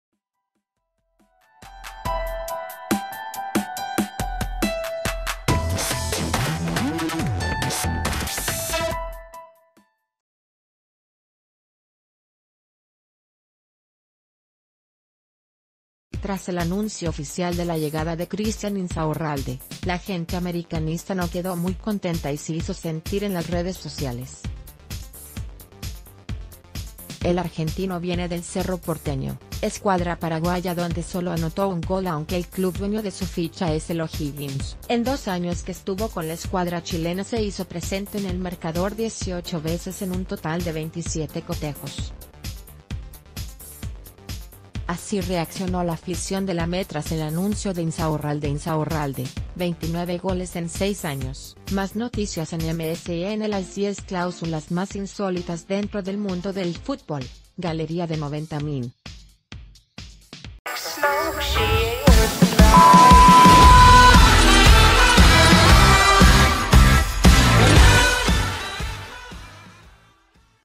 La afición americanista enfurece con el fichaje de Insaurralde
El argentino viene del Cerro Porteño, escuadra paraguaya donde solo anotó un gol aunque el club dueño de su ficha es el O'Higgins. En dos años que estuvo...